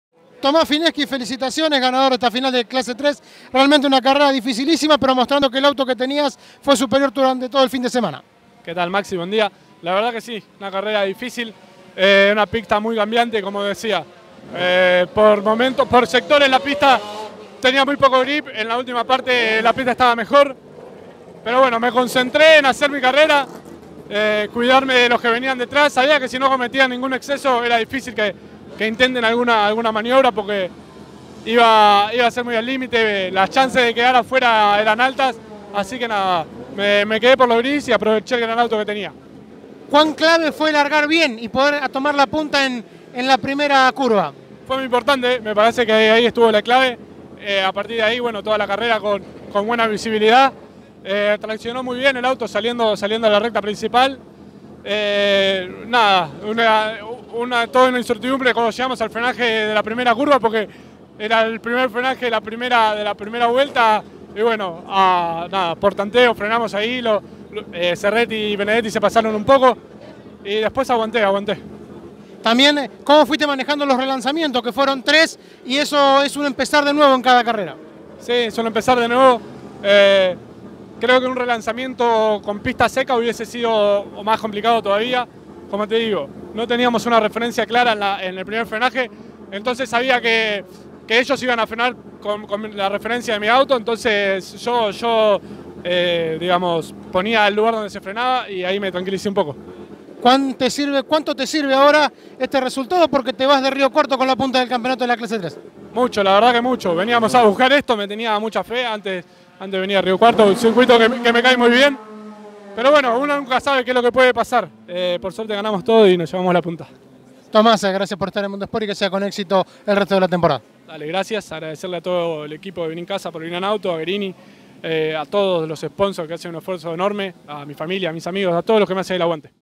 Tomás Fineschi - Victoria Clase 3 Turismo Pista Río Cuarto 2019
Luego de quedarse con la final de la Clase 3 del Turismo Pista en el autódromo de Río Cuarto, Tomás Fineschi pasó por los micrófonos de Mundo Sport y comentó sus sensaciones.